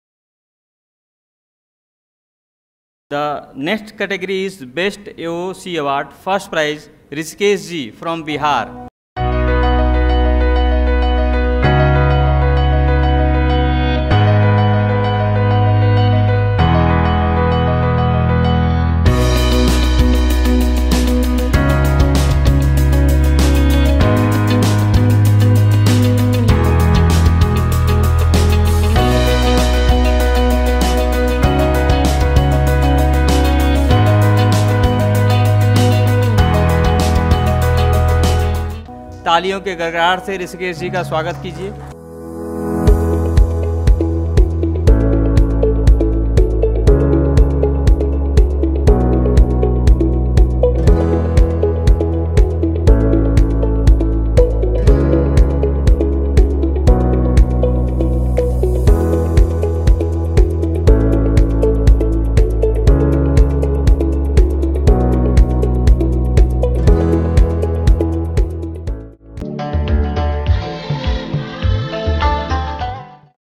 Best Aqua One center Award | Coffed, Patna | World Fisheries Day 2019
Government of India Ministry of Fisheries Animal Husbandry and Dairying Department of Fisheries the Celebrations of WORLDFISHERIESDAY2019, at A.P. Shinde Symposium Hall, NASC Complex, Pusa, New Delhi
Shri Giriraj Singh Hon'ble Union Minister of Fisheries, Animal Husbandry and Dairying
Dr.Sanjeev Kumar Balyan Hon'ble Minister of State for Fisheries, Animal Husbandry and Dairying
Shri Pratap Chandra Sarangi
Hon'ble Minister of State for Fisheries, Animal Husbandry and Dairying